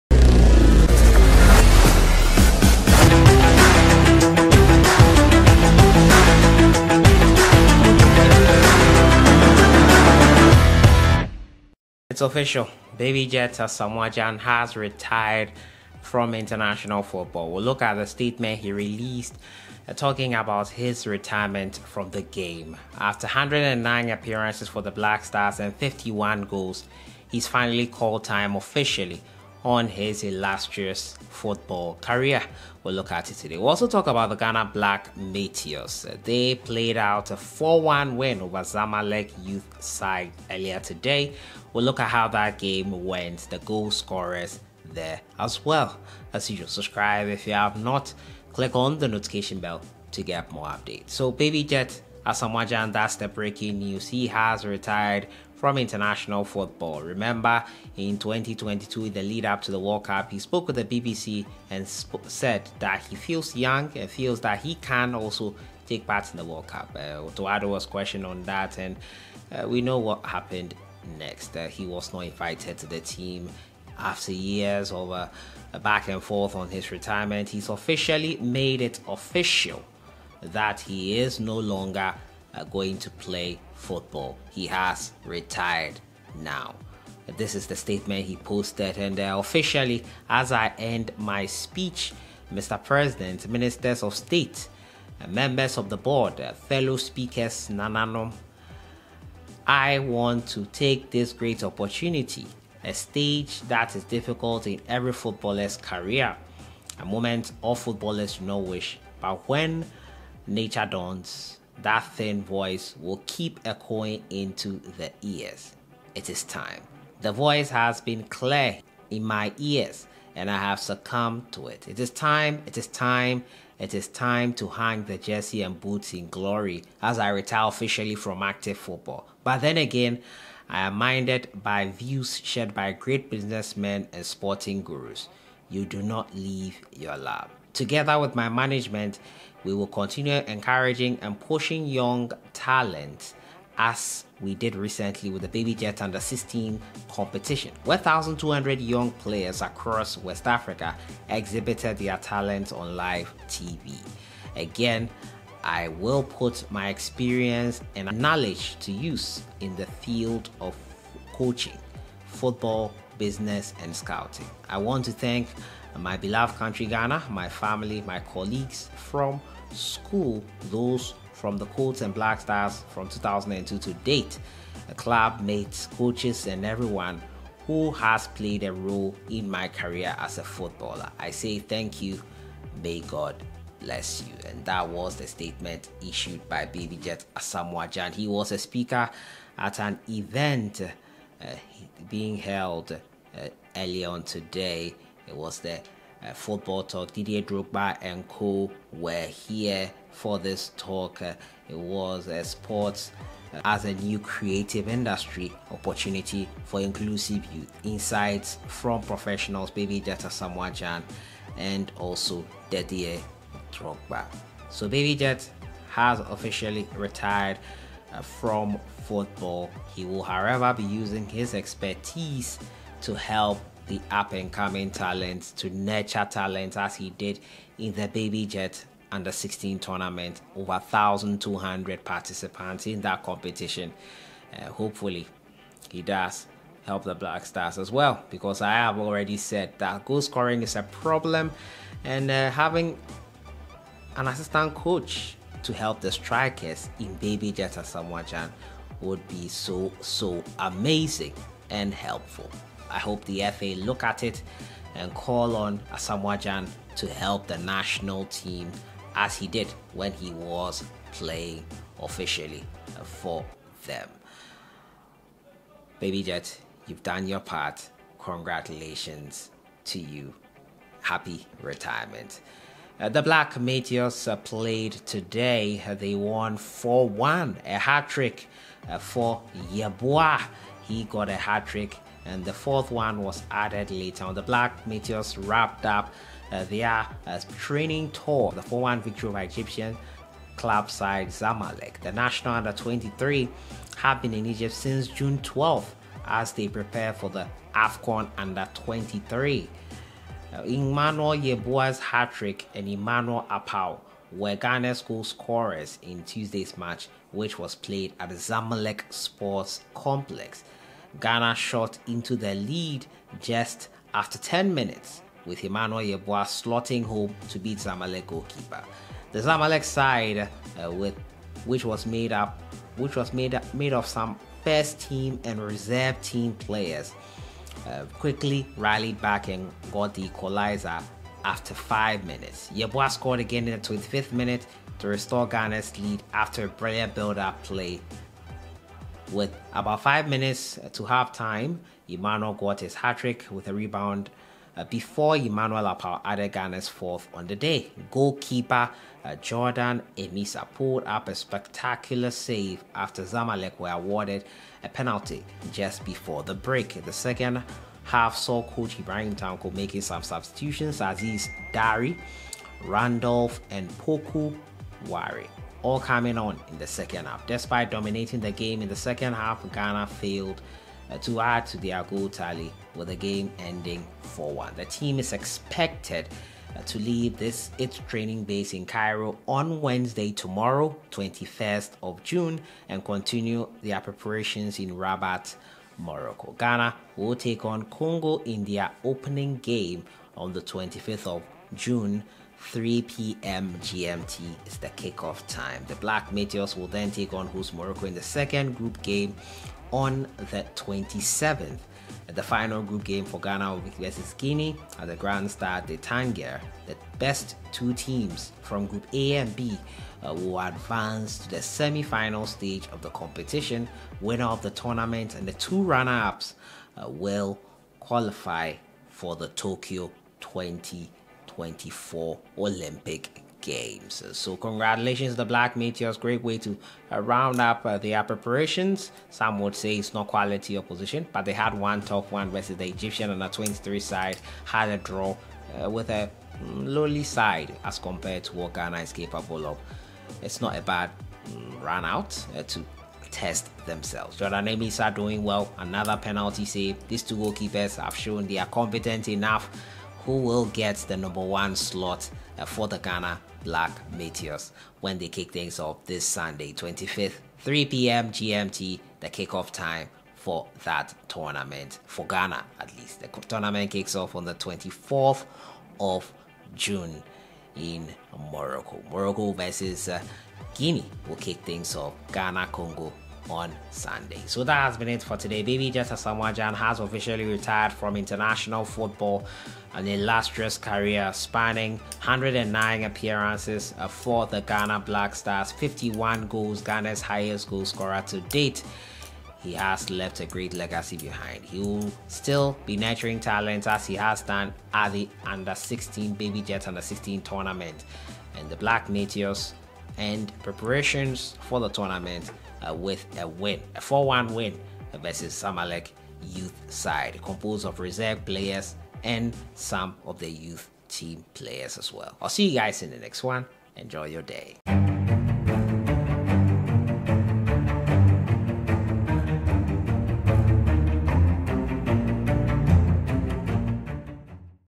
BLACK STARS LEGEND ASAMOAH GYAN RETIRES FROM FOOTBALL
ASAMOAH GYAN FINALLY HAS ANNOUNCED HIS RETIREMENT FROM FOOTBALL